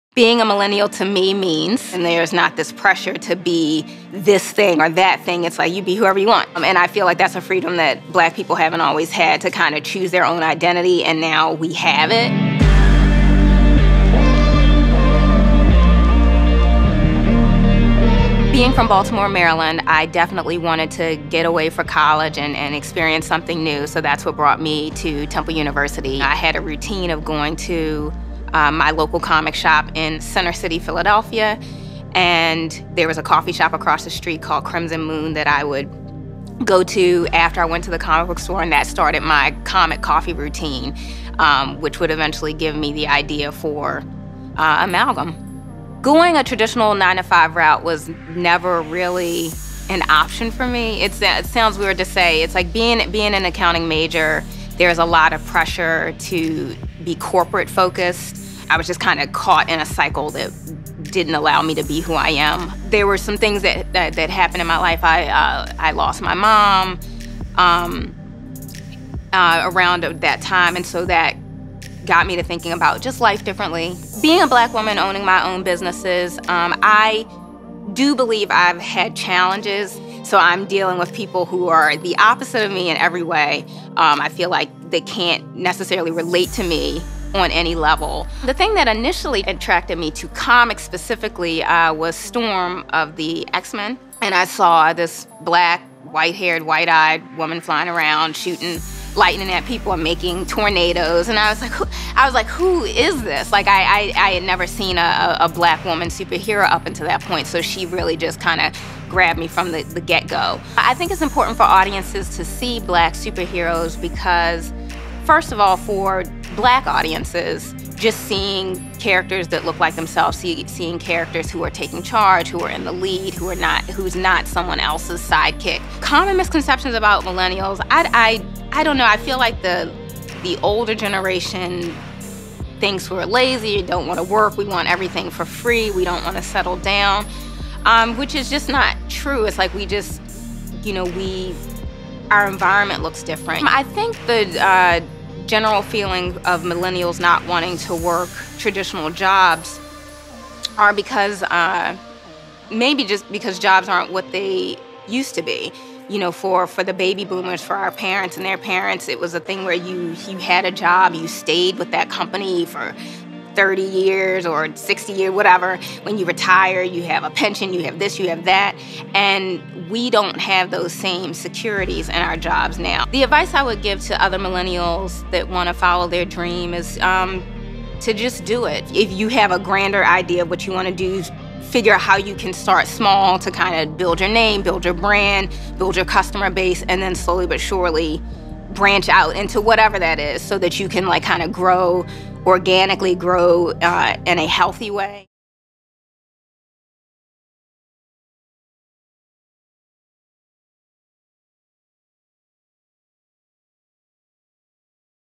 Comic Book Store Owner Ariell Johnson Talks Her Love of Comics | Millennials Making Moves
Ariell Johnson is the first black woman to own a comic book store on the east coast.

Millennials are making moves and Ariell Johnson is one of them. The Temple University graduate is the Founder and President of Amalgam Comics & Coffeehouse, Inc. in Philadelphia, PA, which is the first Black-owned comic store on the East Coast. For the 34-year-old, her hybrid shop is an extension of the need for more representation of African Americans in all facets of life, including the comic book industry.